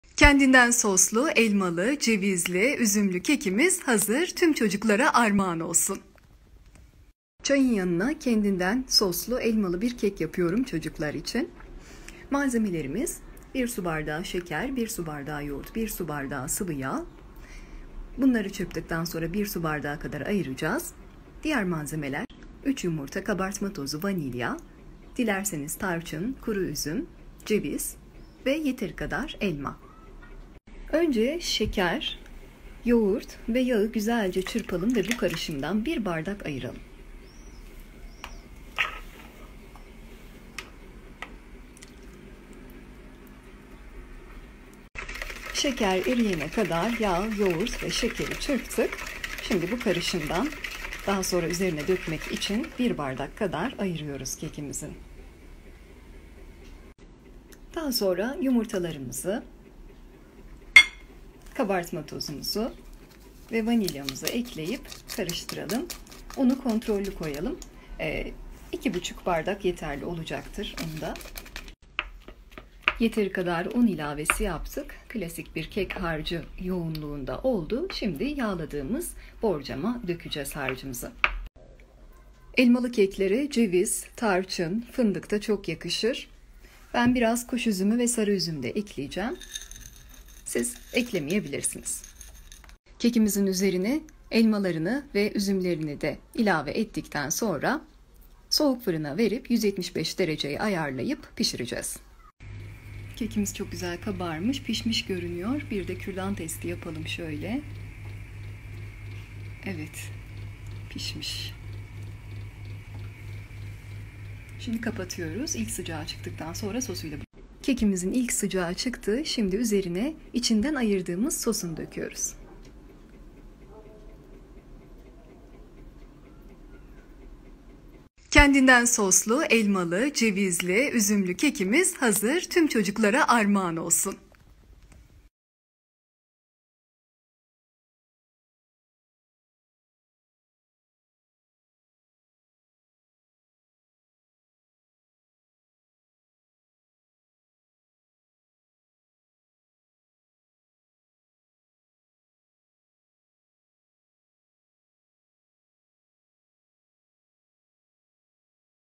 Kendinden soslu elmalı kek
Kendinden soslu Elmalı tarçınlı cevizli üzümlü enfes kek 👌😋 @nefisyemektarifleri 🇹🇷🇹🇷🇹🇷🇹🇷🇹🇷🇹🇷

malzemeler

3 yumurta
1 sb şeker
1 sb yoğurt
1 sb sıvıyağ
1 vanilya
1 kabartma tozu
2,5 sb un

Üzeri için;
3 elma
Tarçın
1 fincan ceviz içi
1 fincan üzüm

Servis için; pudra şekeri

Yağ , yoğurt ve şekeri çırpalım, bu karışımdan bi bardak ayıralım , diğer kek malzemelerini karıştıralım yağlanmış borcama dökelim , üzerine dilimlenmiş elmaları dizelim , tarçın ekelim , isteğe göre ceviz içi ve üzüm ekelim , soğuk fırına koyup 175 dereceye ayarlayalım, kürdan testi yapıp pişince çıkaralım, ilk sıcağı çıkınca ayırdığımız karışımı dökelim , 1 saat sonra dilimleyip pudra şekeri ile servis edelim🤪 afiyet şifa olsun kilo olmasın, doğru saatte doğru miktarda tüketelim bol hareket edelim, israf etmeyelim, ikram edelim, artanları hayvan dostlarımızla paylaşalım 😉